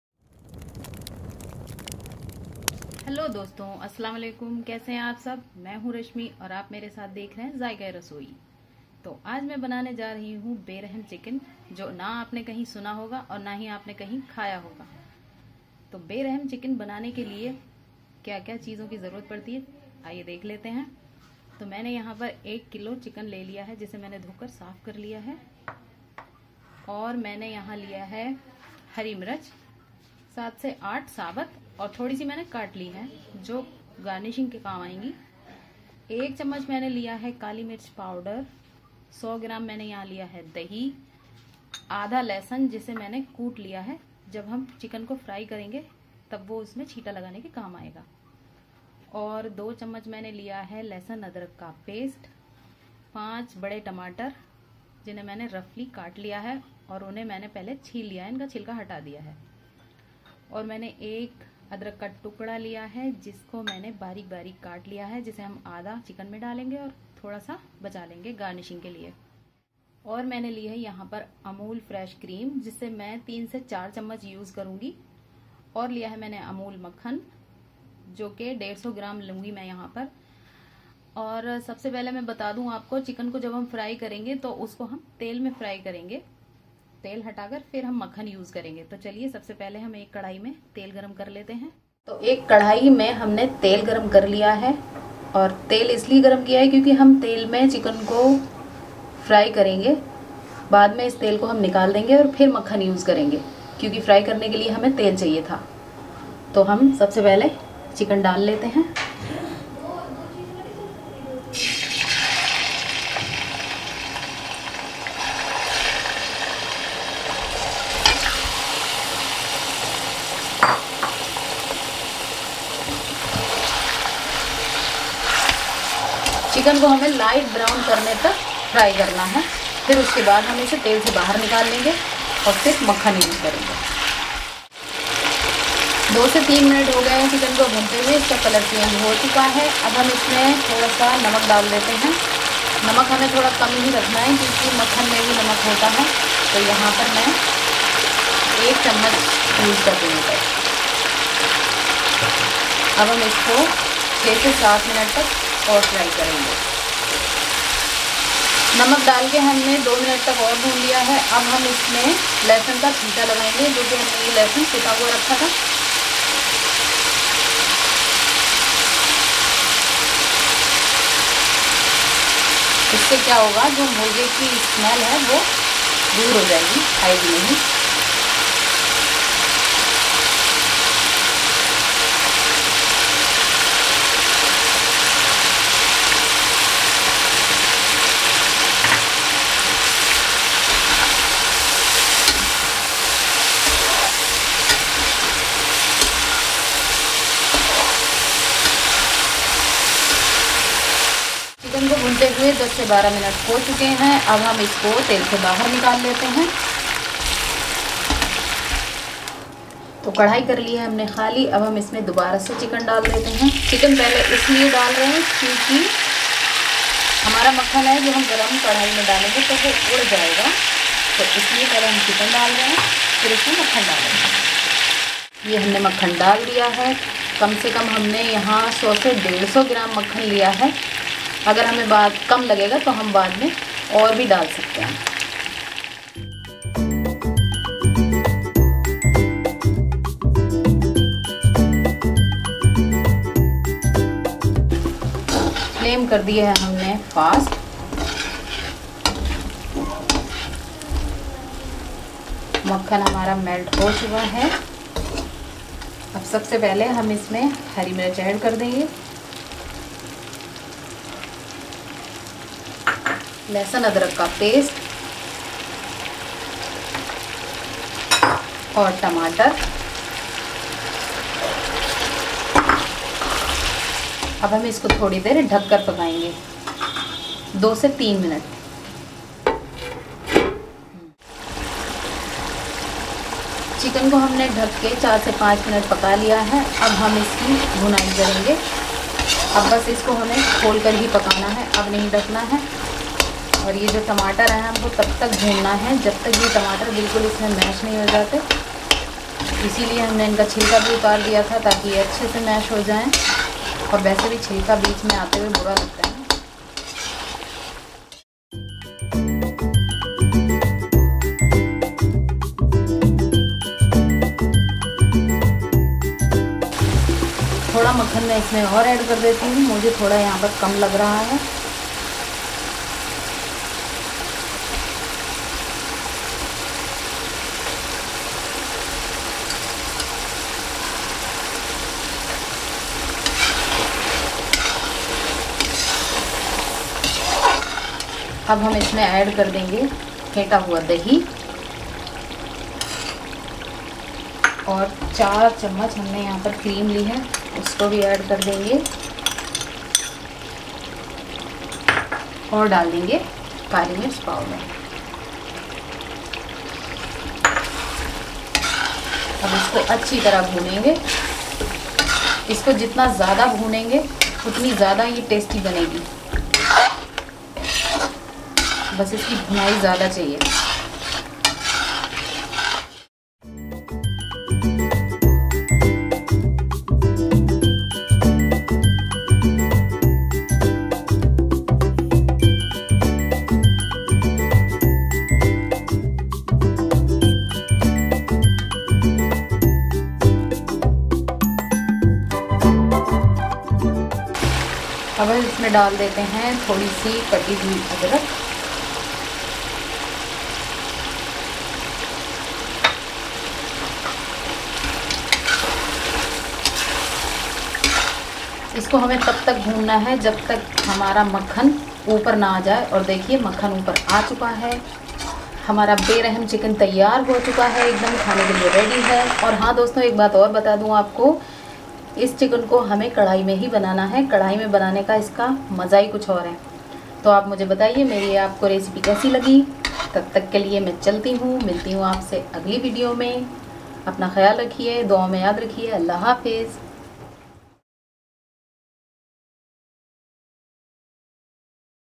Beraham Chicken || Youtube Par Pehli Bar
Hello Dosto,
Aaj hum banayenge "CHICKEN BERAHAM" jo apne pehle kabhi na hi dekha hoga aur na hi khaya hoga. Ek bar banakr dekho

SHUKRIYA..